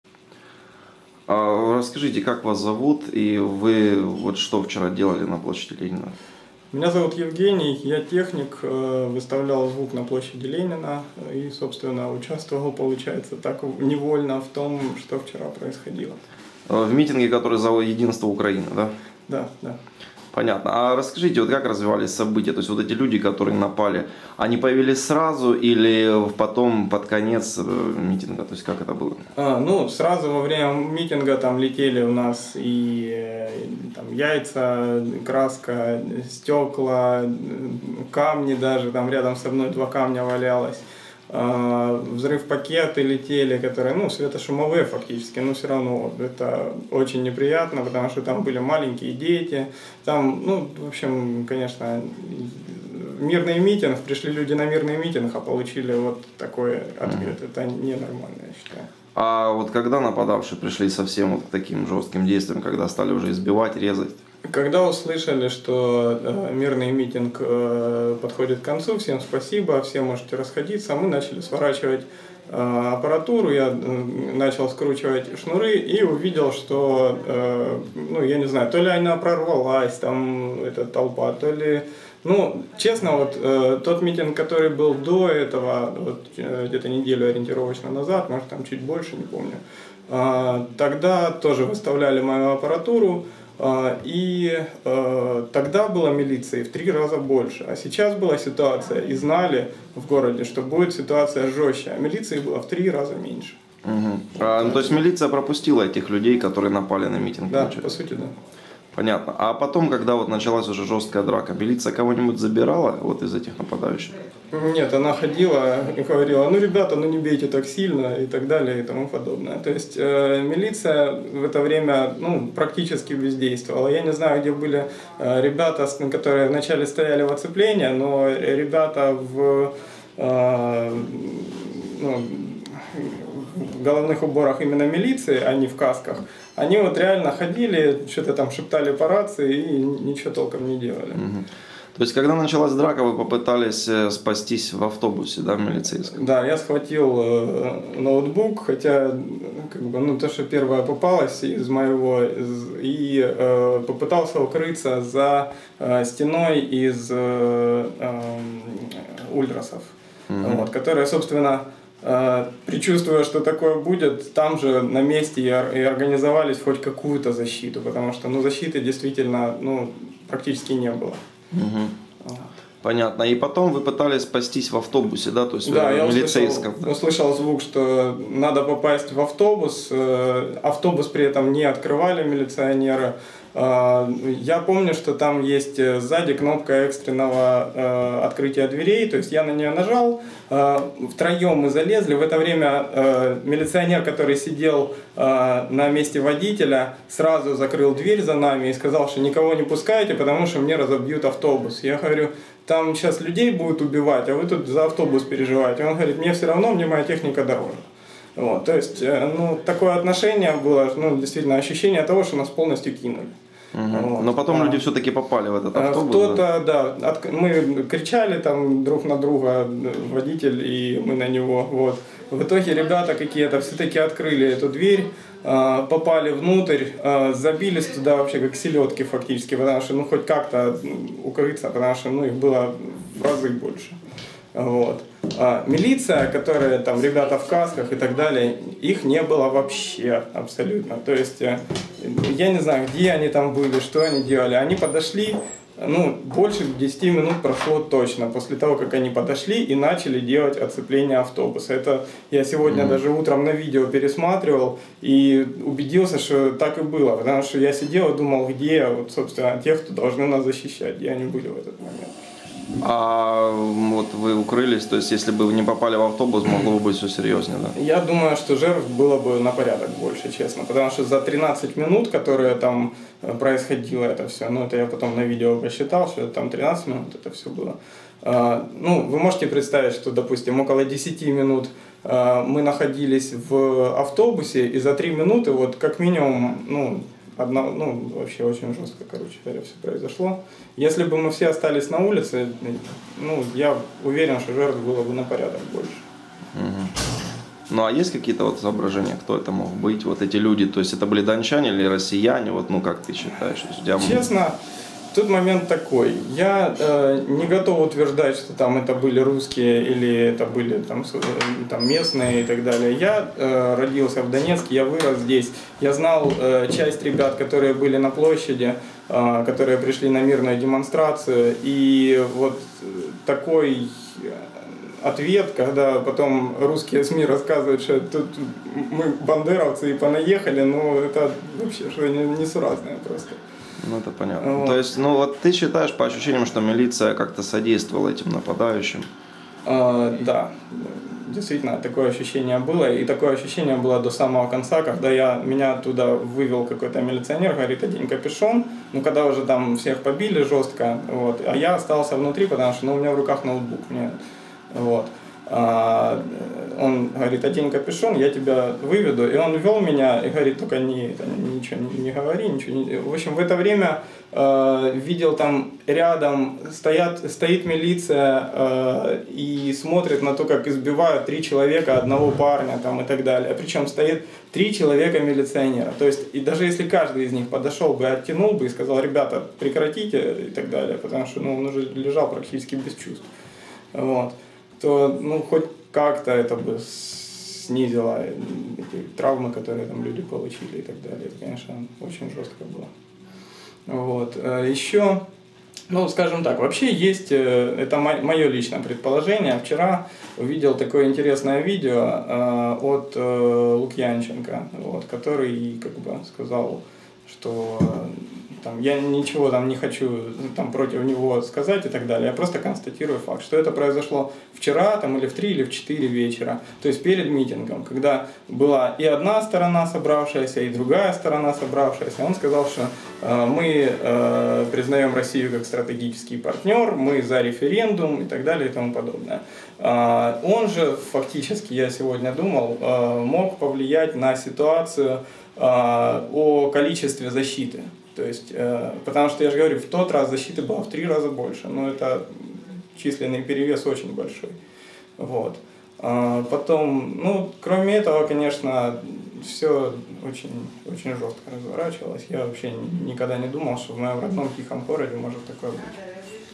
Как милиция пособничала избиению людей в Донецке
Участник митинга за единство Украины рассказывает, как пророссийские боевики избивали граждан под прикрытием милиции